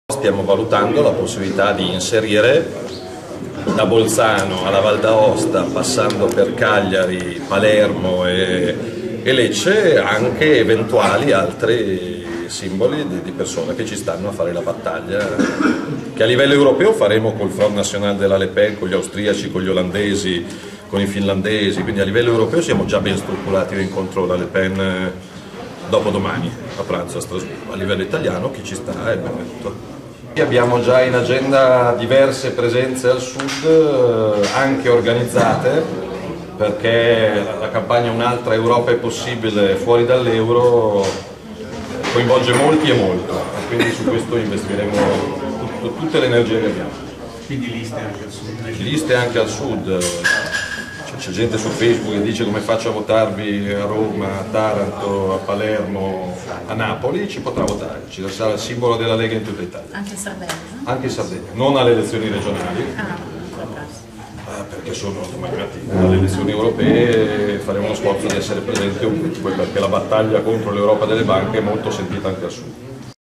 Lega, Salvini: "Liste anche al Sud per le ...
Il segretario della Lega Nord, Matteo Salvini, annuncia che "ci saranno liste anche al Sud per la battaglia sull'Europa" in occasione delle elezioni europee a maggio. "Stiamo valutando la possibilità di inserire eventuali altri simboli, da Bolzano a Palermo, - dice Salvini - sono già in agenda diversi impegni e appuntamenti organizzati al Sud"
(di Ivano Pasqualino)